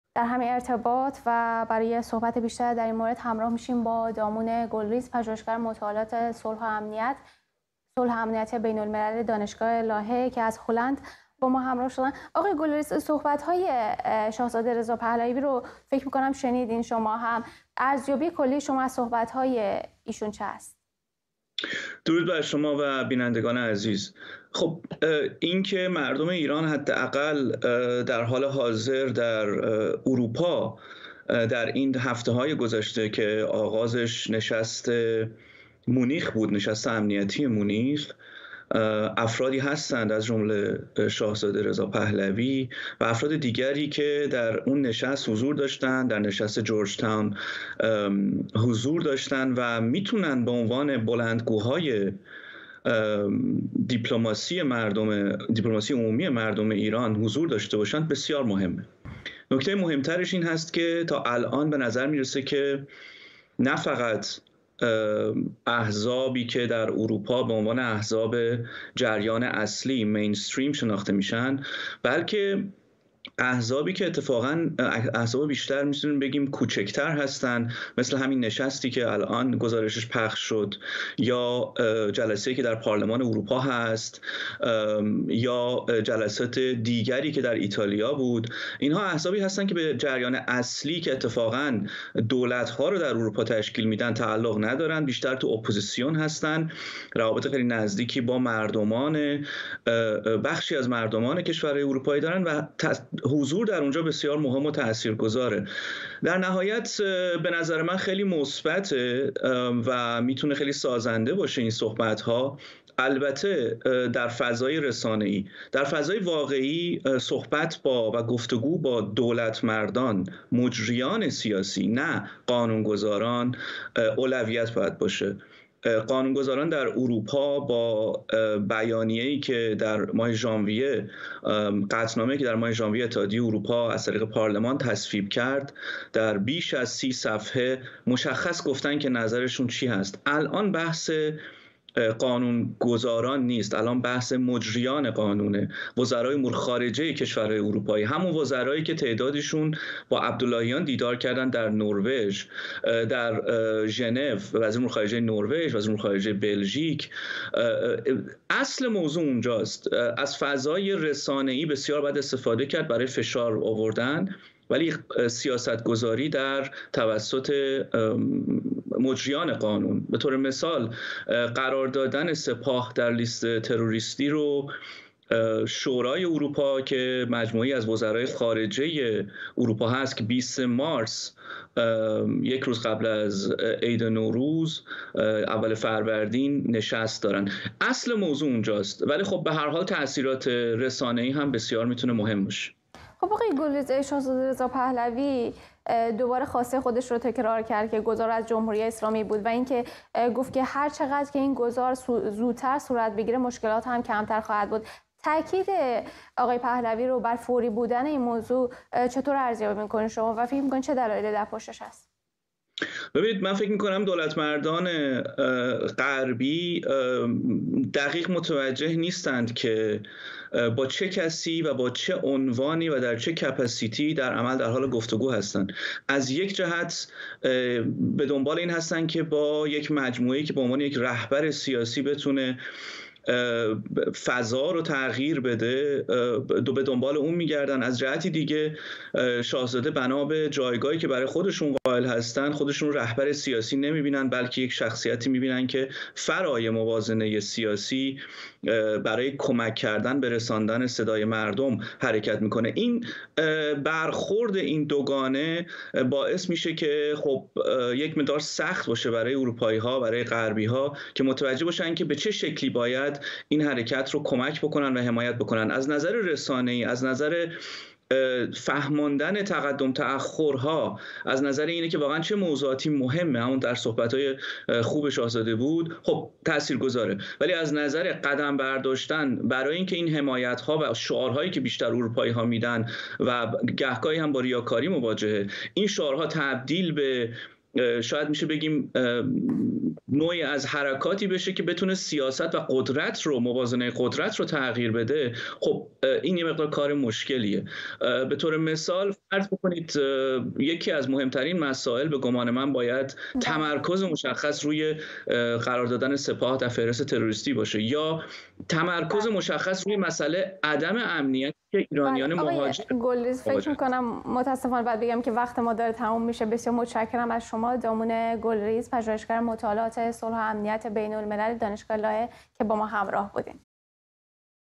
ارزیابی دامون گلریز از جلسه شاهزاده رضا پهلوی با اعضای پارلمان بلژیک
جلسه شاهزاده رضا پهلوی با اعضای پارلمان بلژیک

دامون گلریز، پژوهشگر مطالعات صلح و امنیت بین‌الملل: جلسات شاهزاده رضا پهلوی می‌توانند در فضای رسانه‌ای مفید و سازنده باشند. اما در فضای واقعی، گفت‌وگو با دولت‌مردان و مجریان سیاسی و نه قانون‌گذاران، باید در اولویت باشد.